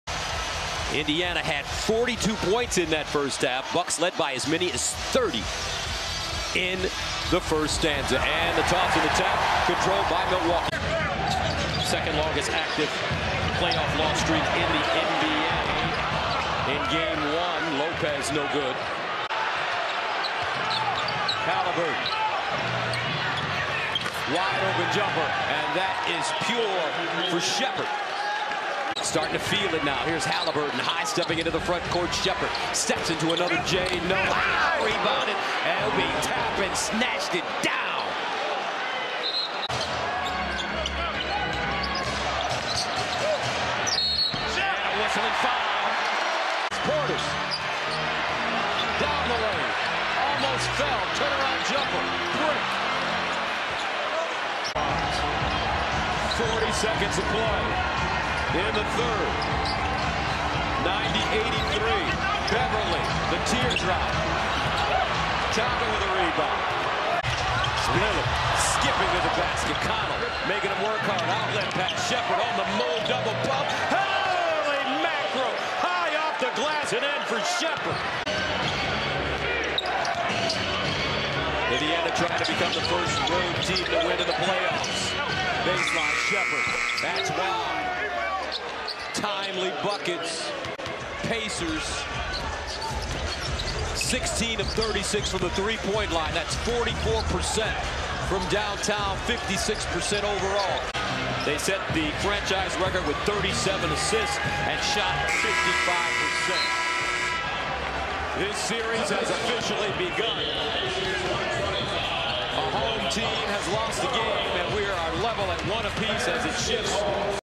Ben Sheppard 8 pts 3 reb 1 stl vs Milwaukee Bucks | 2024-04-23
Ben Sheppard Highlights from Indiana Pacers vs Milwaukee Bucks on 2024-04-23.

Player Stats:
23:25 mins
8 Points
3 Rebounds (0 Offensive)
0 Assists
1 Steals
0 Blocks
0 Turnovers
3/4 Field Goal
0/0 Free Throw